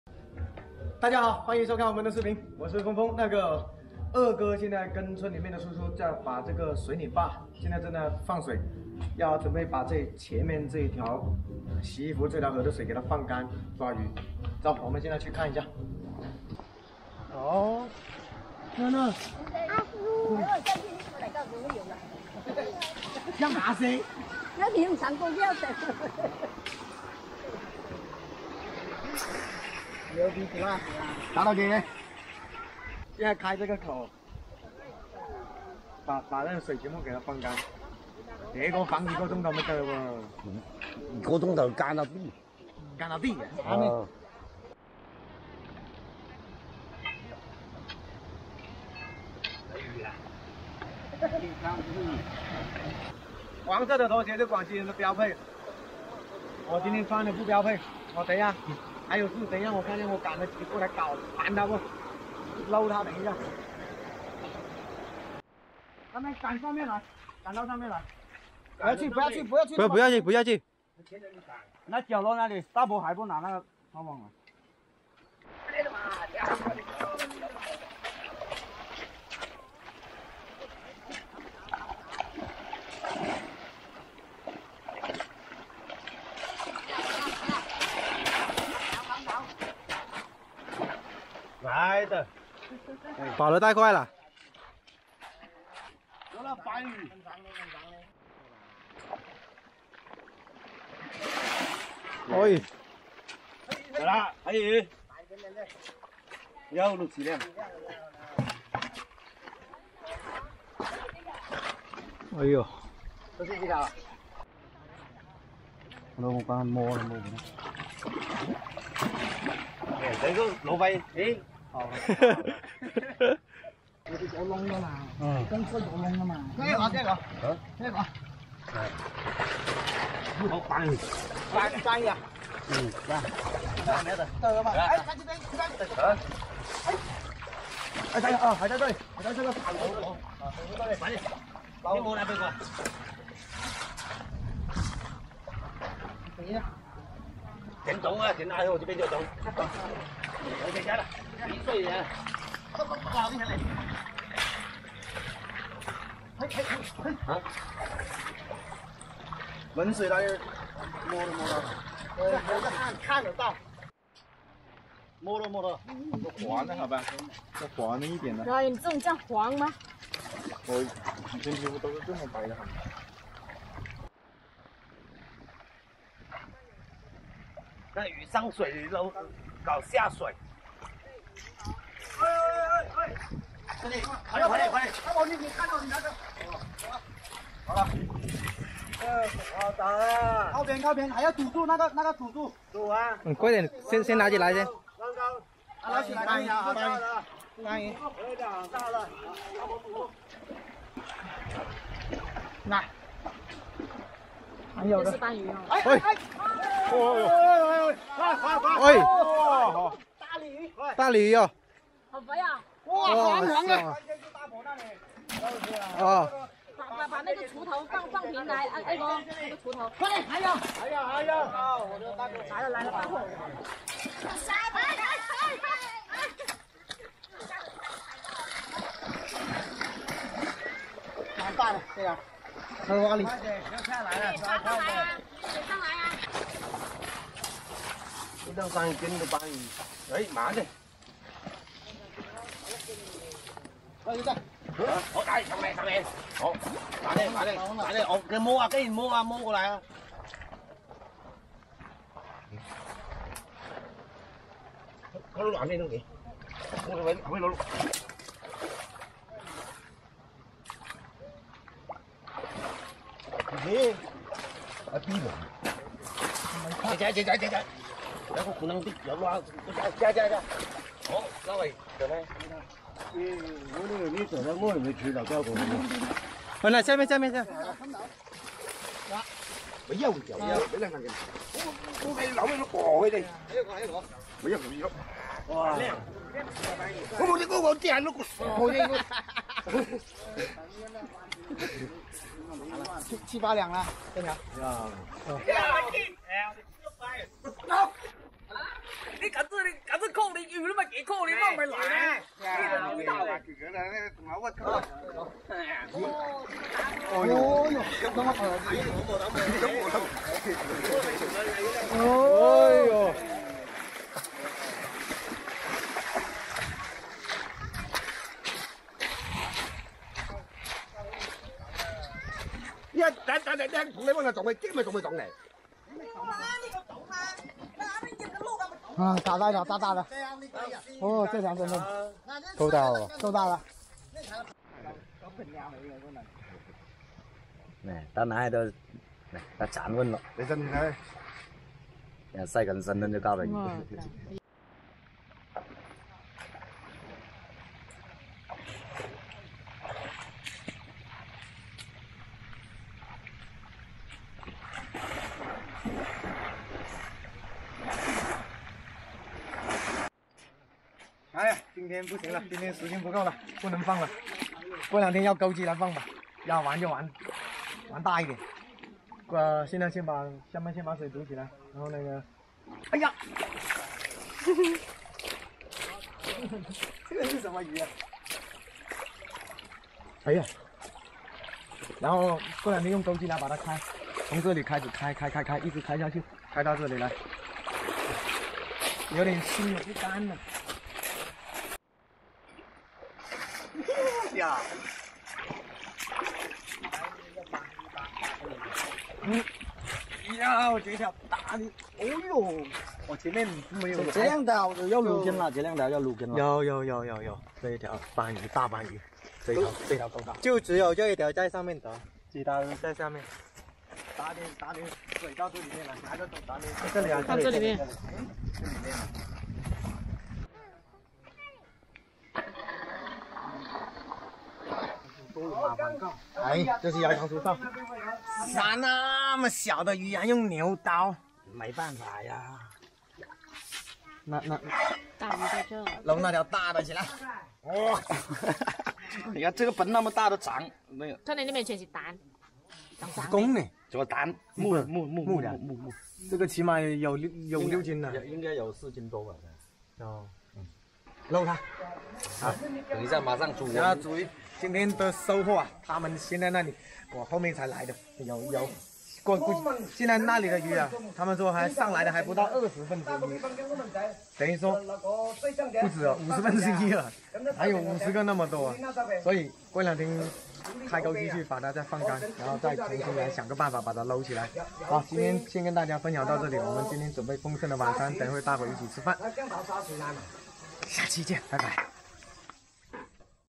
枫枫和二哥玩大了，把村里几十年大坝放干水，村民一起抓鱼好刺激【农人DJ枫枫】
农人DJ枫枫 农村生活 捕鱼 本视频是一部原创拍摄的短剧，完全符合YouTube社区准则。